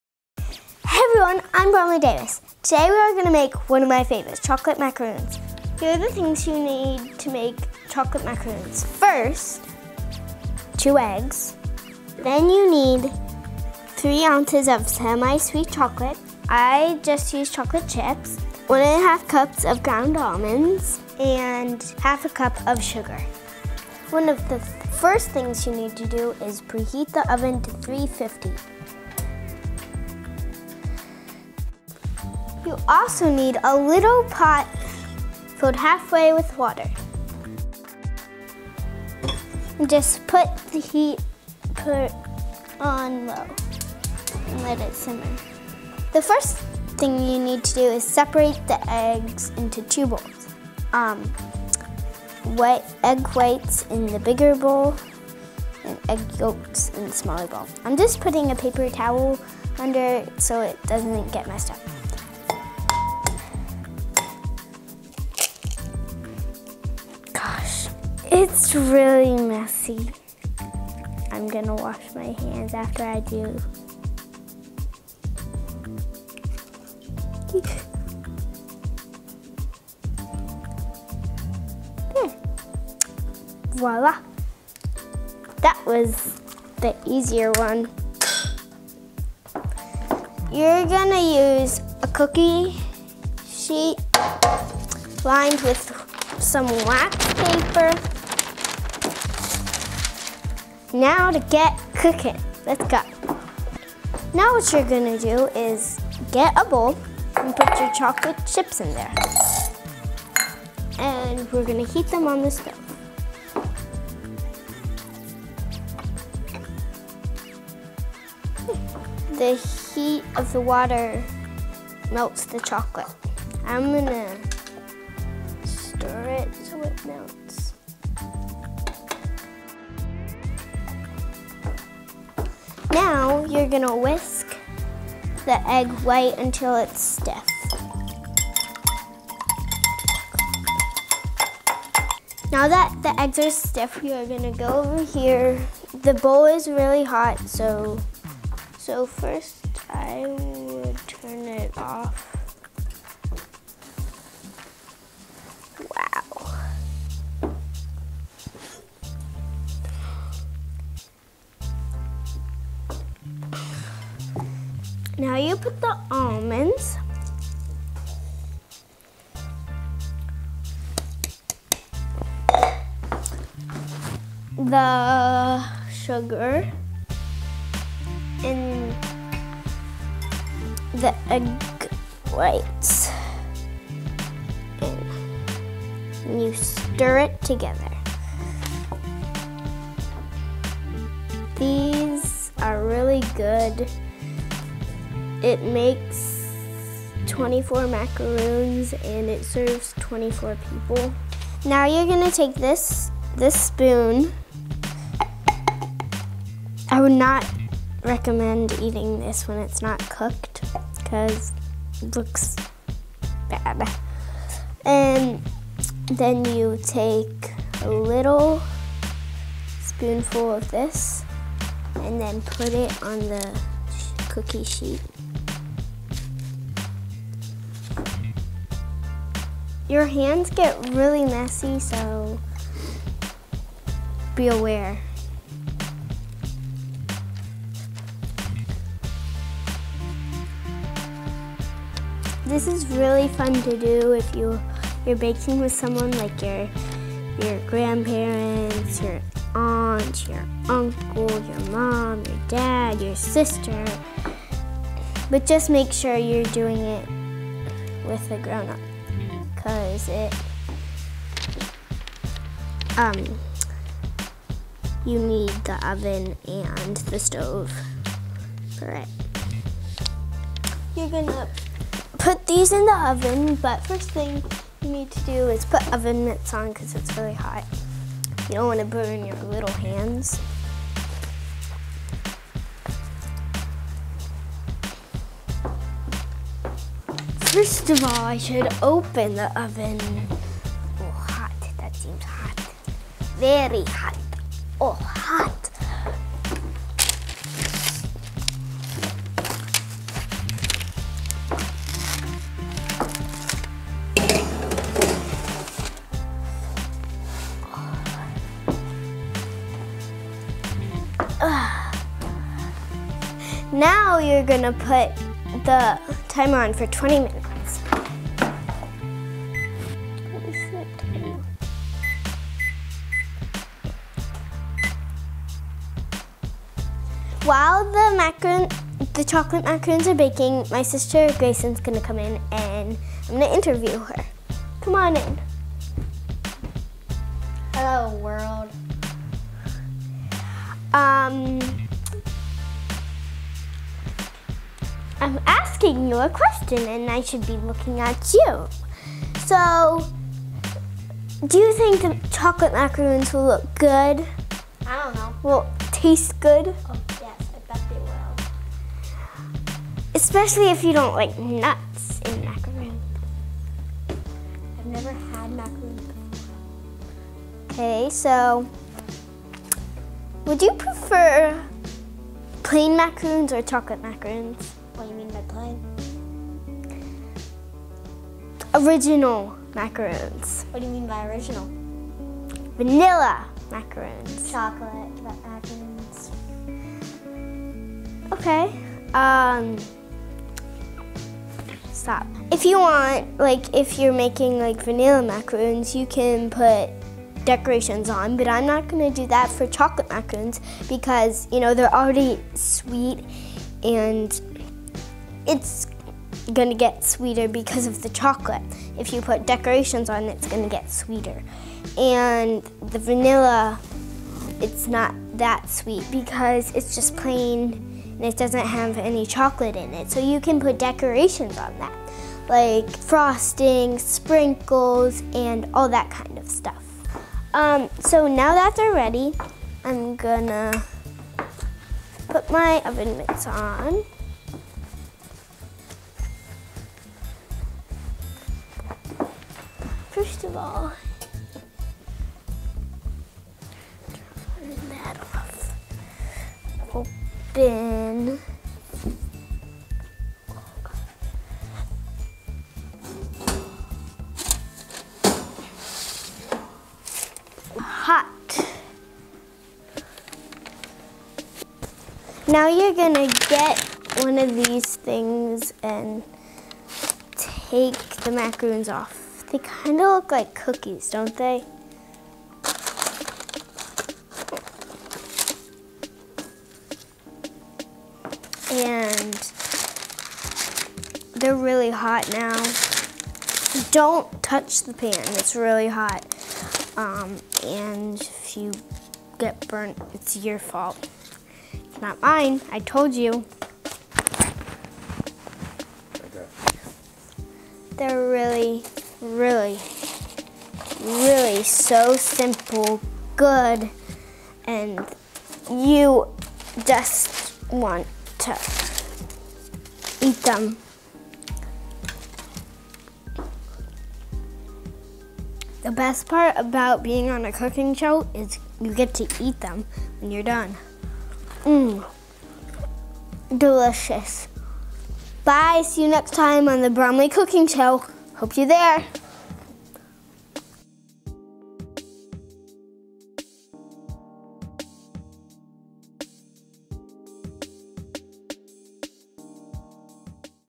Bromley's Cooking Show - Pilot Episode
Bromley shows how to make chocolate macarroons